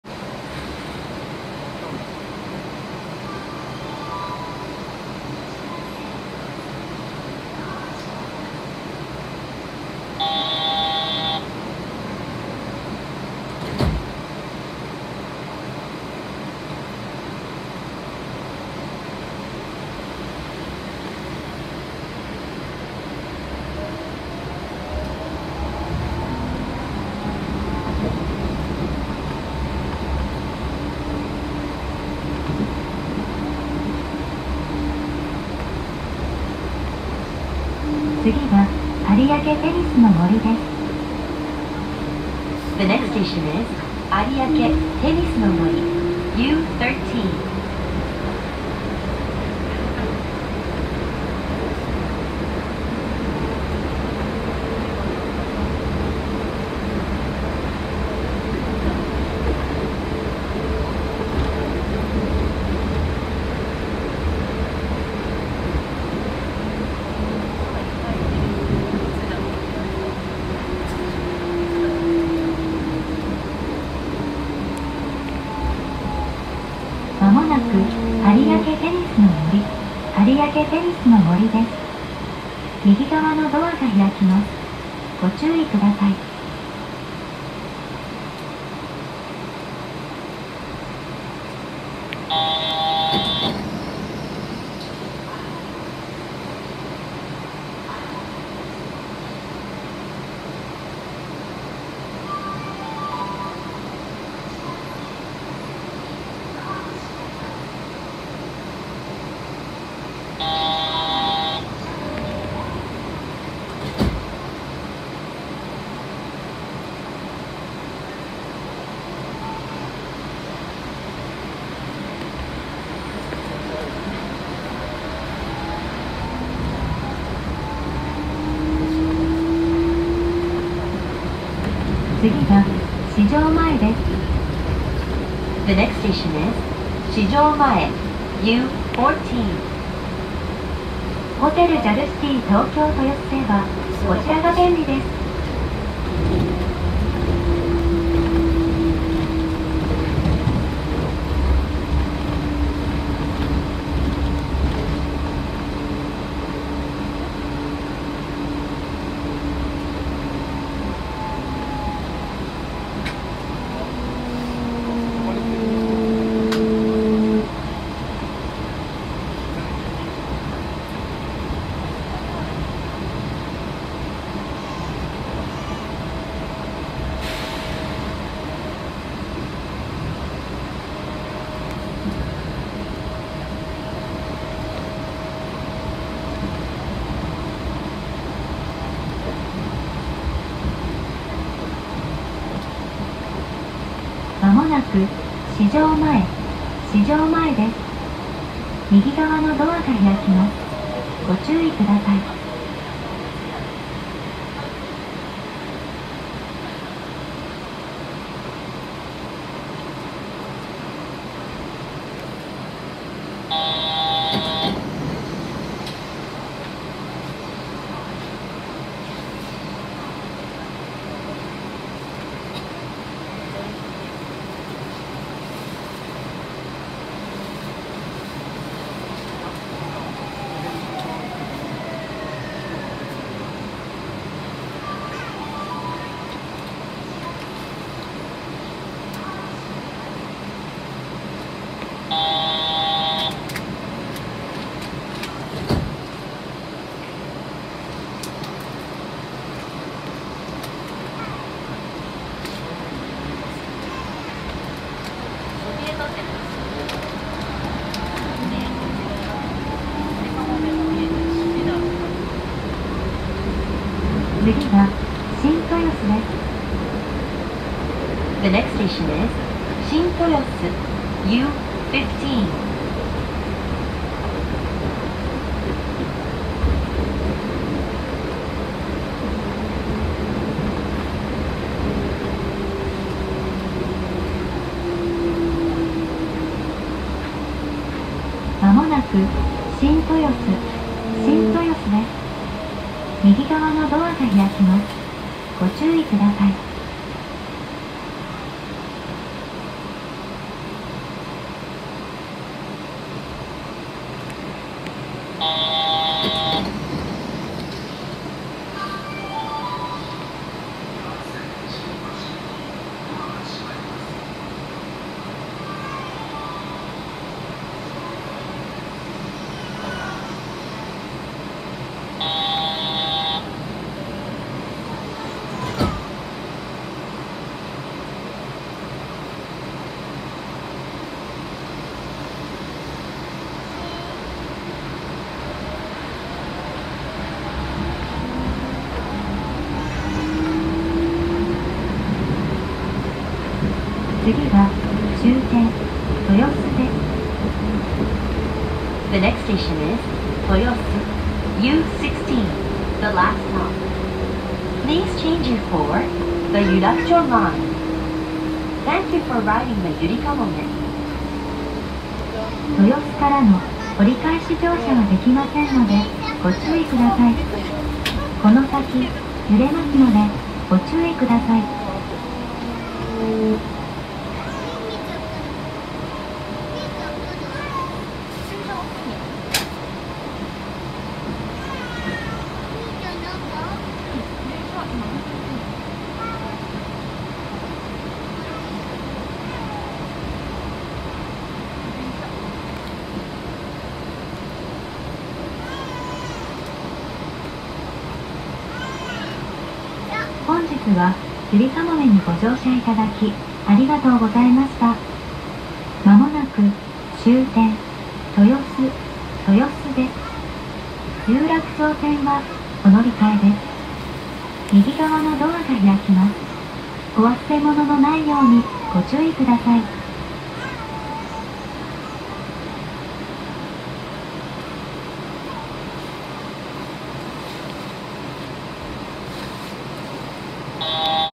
ゆりかもめ有明駅から豊洲駅までの車窓動画です。
ゆりかもめ東京臨海新交通臨海線です。